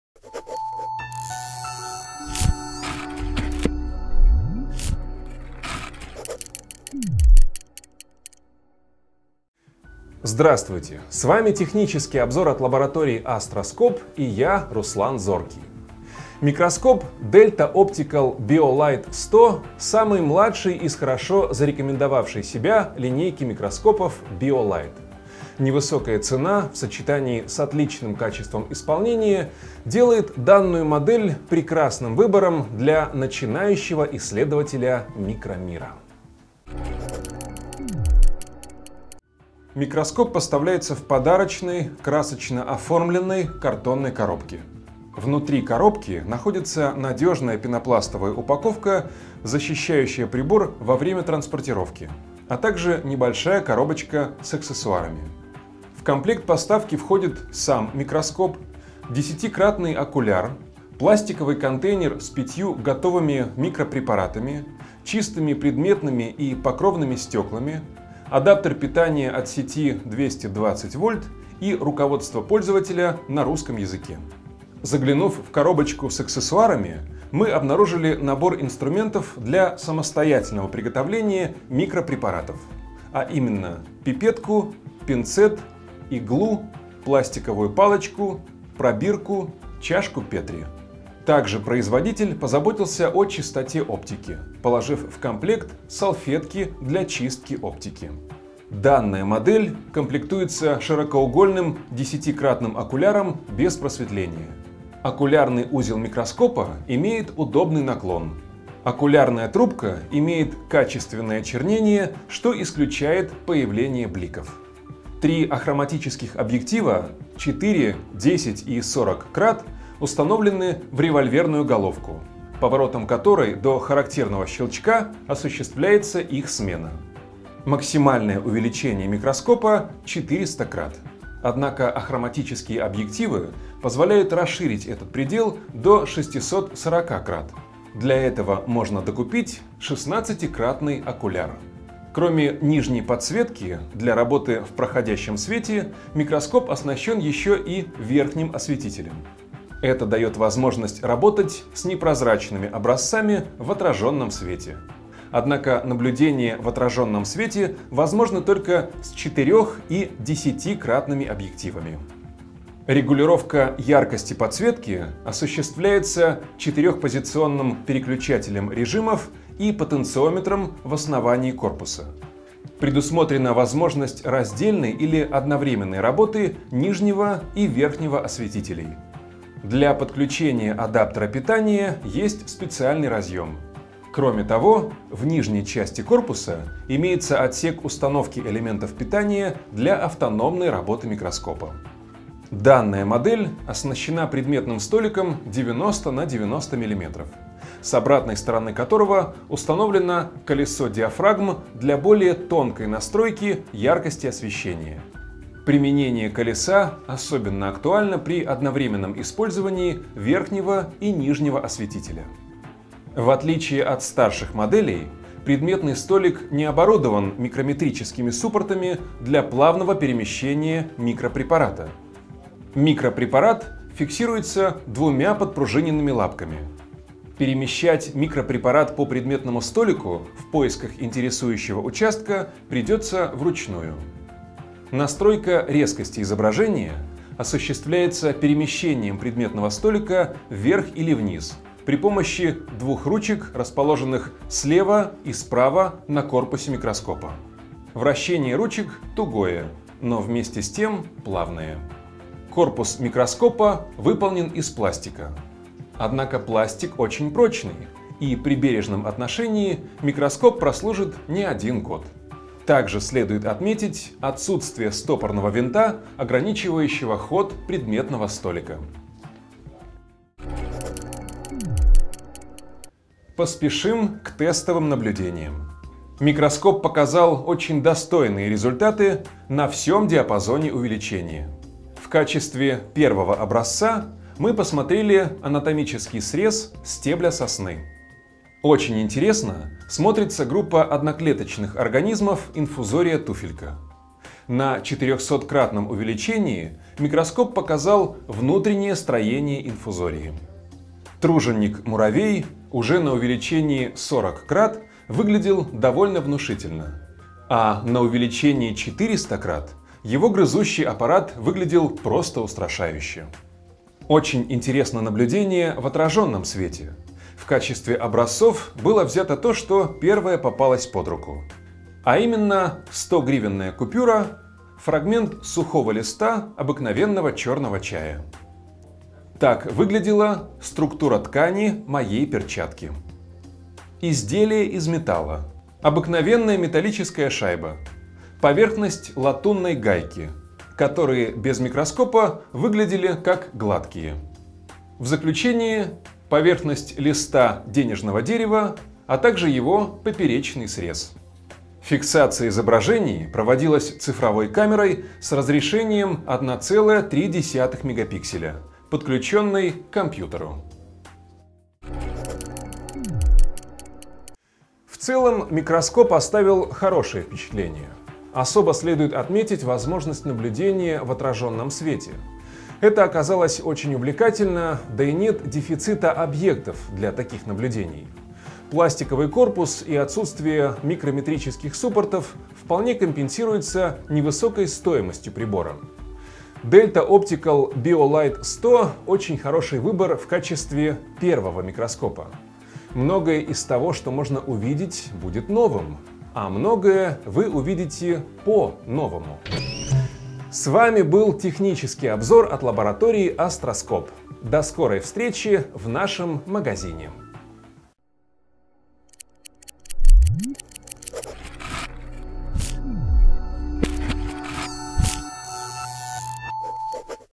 Обзор микроскопа Delt Optikal Biolight 100
Микроскоп Delta Optical Biolight 100 самый простой микроскоп хорошо зарекомендовавшей себя, и получившей большую популярность линейки микроскопов Bioligt.